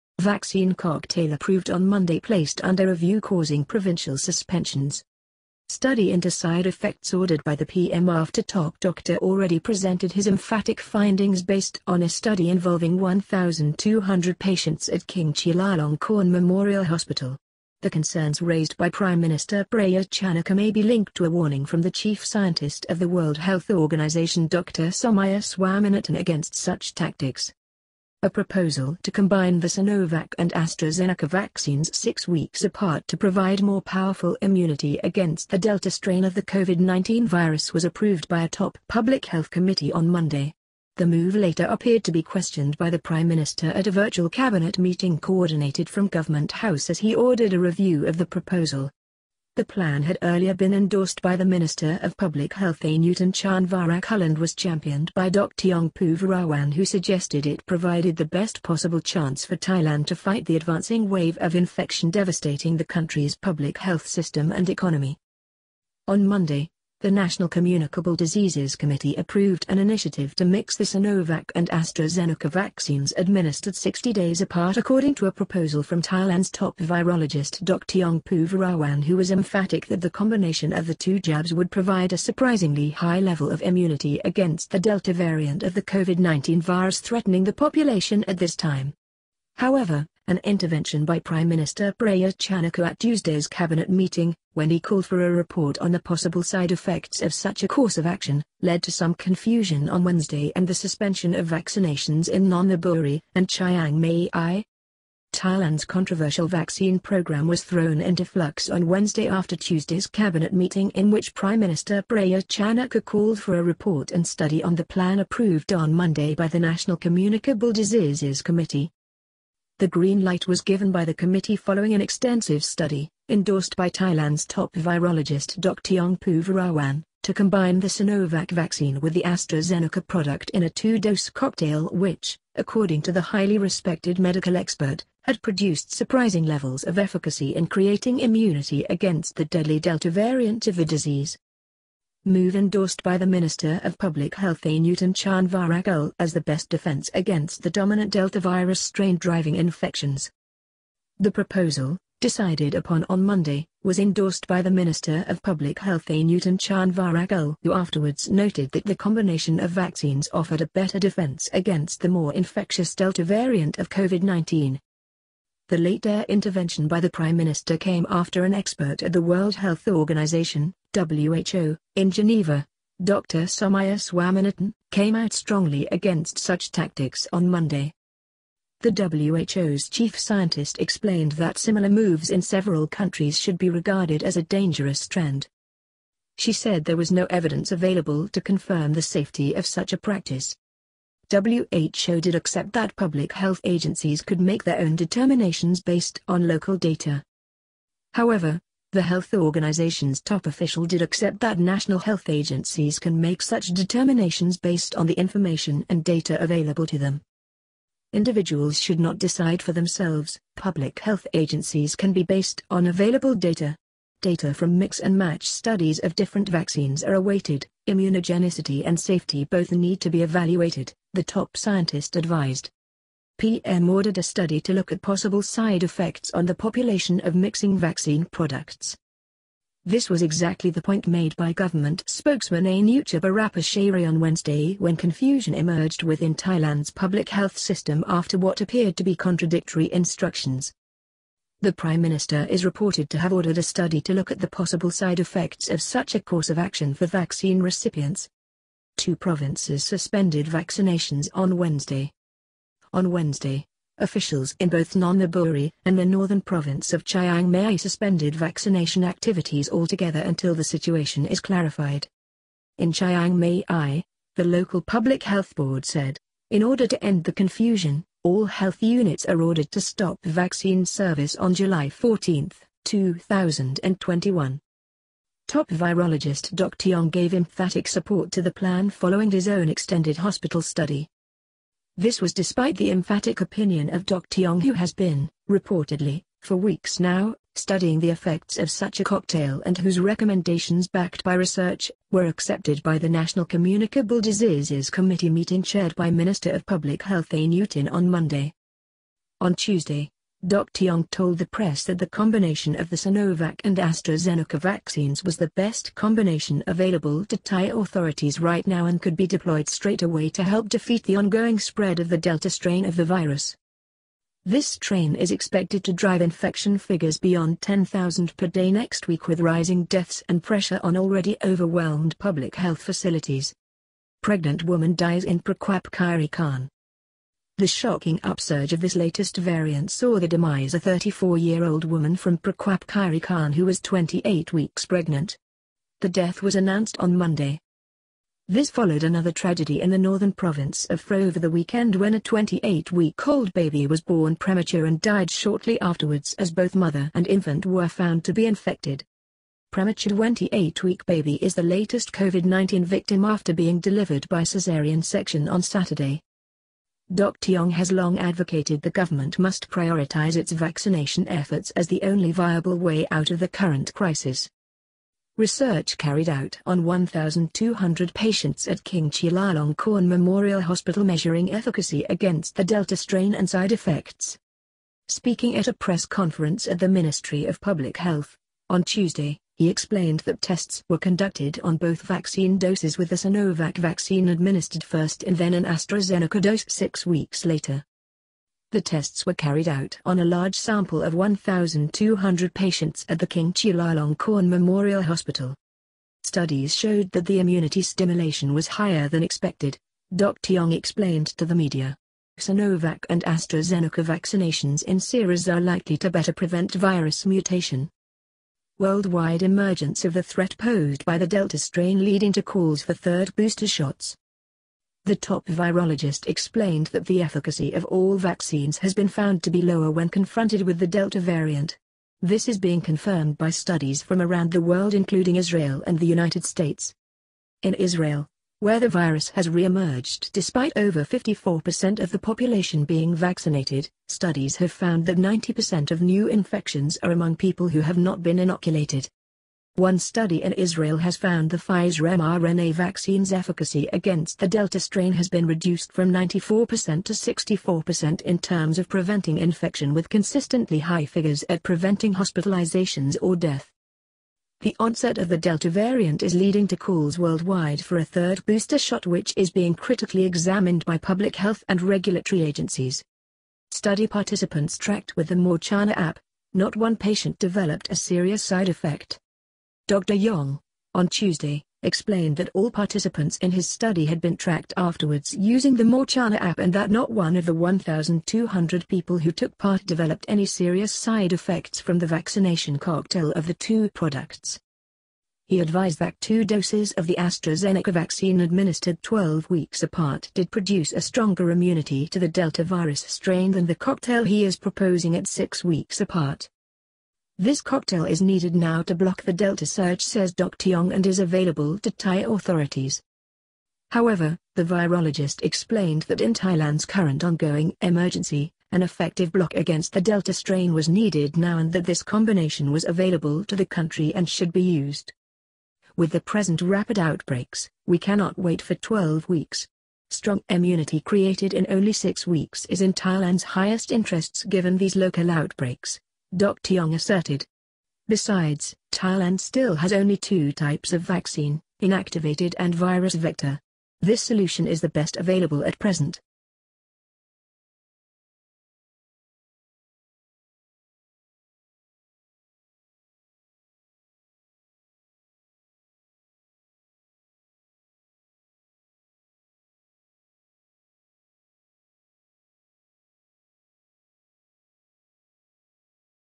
Vaccine cocktail approved on Monday placed under review causing provincial suspensions
Study into side effects ordered by the PM after top doctor already presented his emphatic findings based on a study involving 1,200 patients at King Chulalongkorn Memorial Hospital. The concerns raised by Prime Minister Prayut Chan ocha may be linked to a warning from the Chief Scientist of the World Health Organisation Dr Somaya Swaminathan against such tactics.

Carla Boonkong reports from Bangkok, Nonthaburi and Chiang Mai.